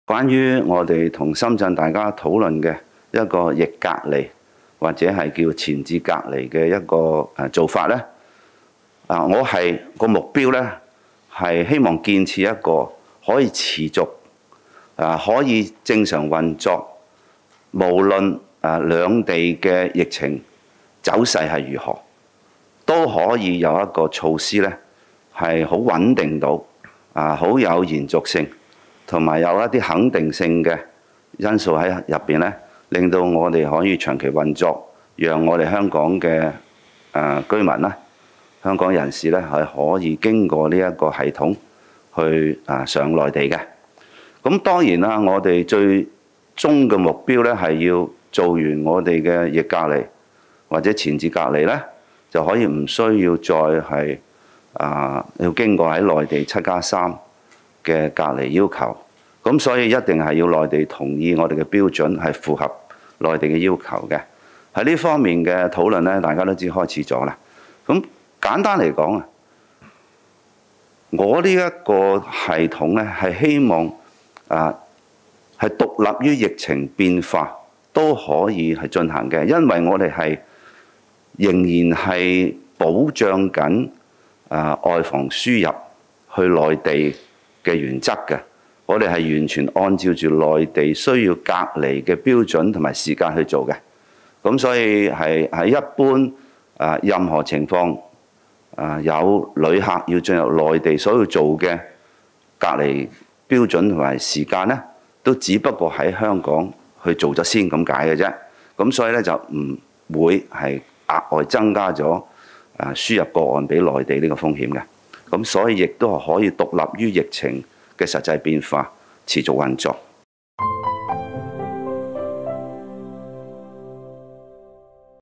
李家超：“逆向隔离”希望是稳定、持续运作的系统
9月13日，香港特区行政长官李家超在记者会上表示，与内地正研究的“逆向隔离”安排希望是一个独立于两地疫情变化的系统，是一个具有稳定性、并且可以长期运作的系统。